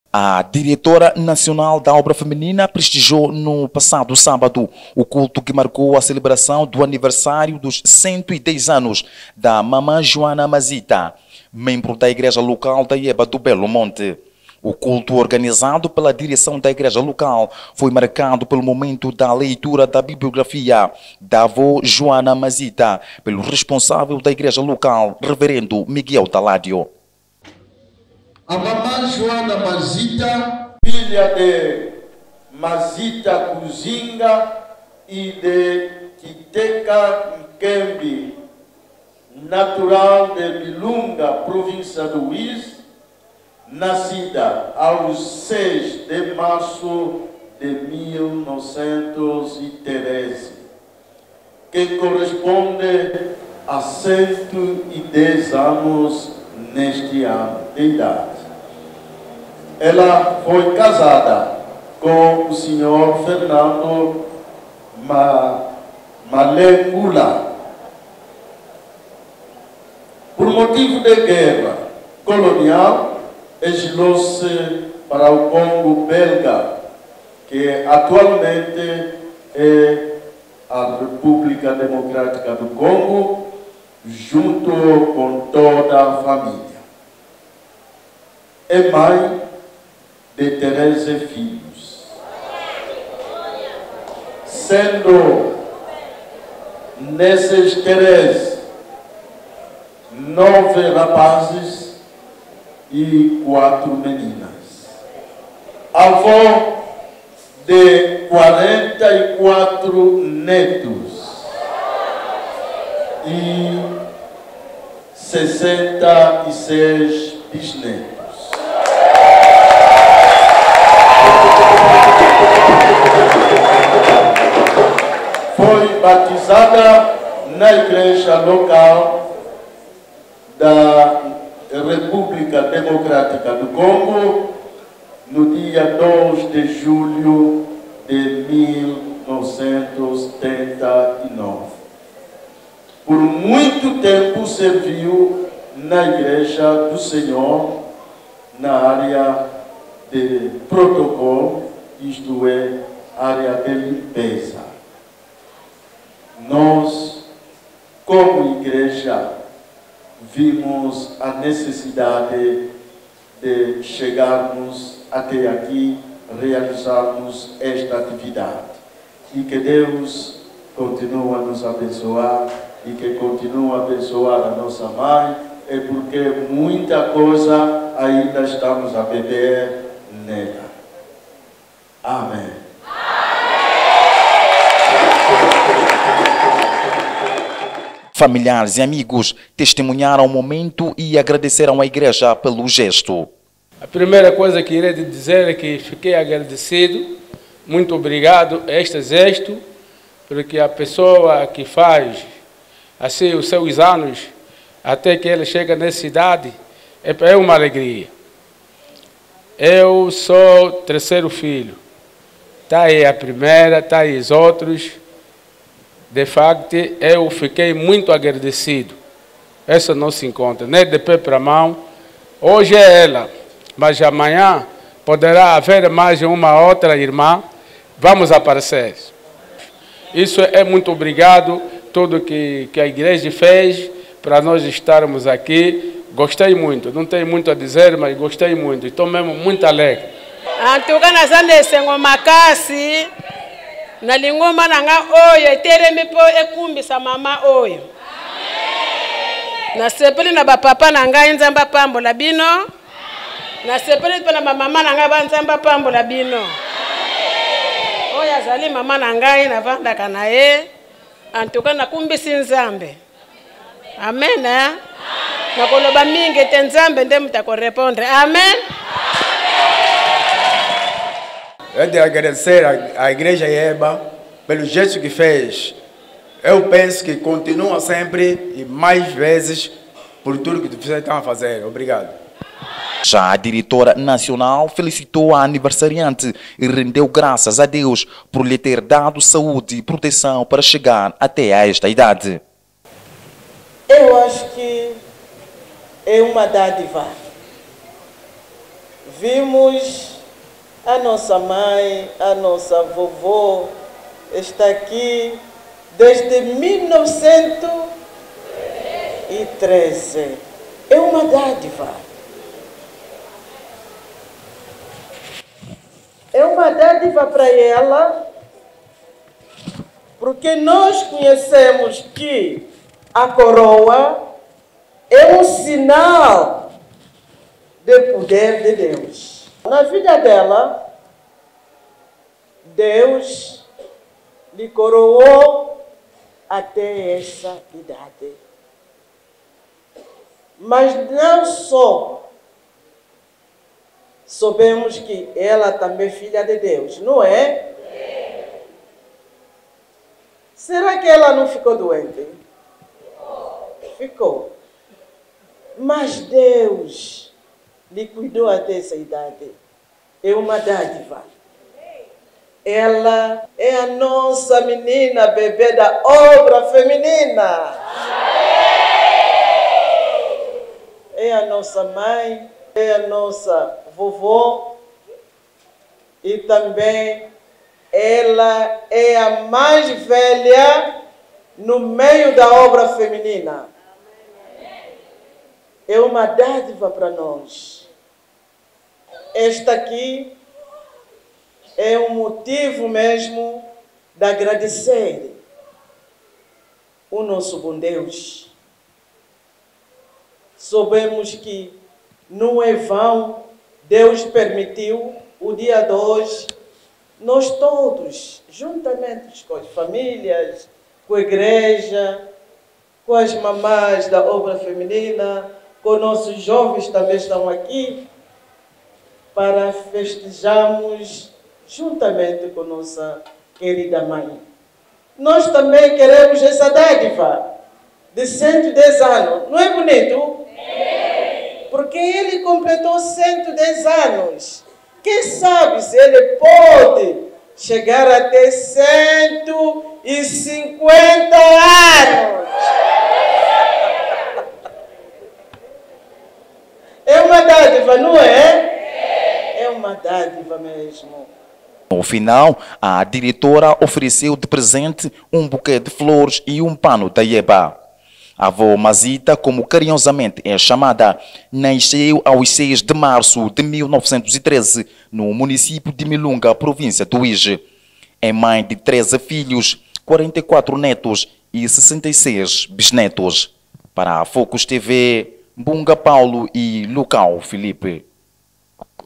A DIRECTORA NACIONAL DA OBRA FEMININA DA IEBA PARTICIPOU NO ANIVERSÁRIO DA MAMÃ JOANA MAZITA.
IGREJA LOCAL DO BELO MONTE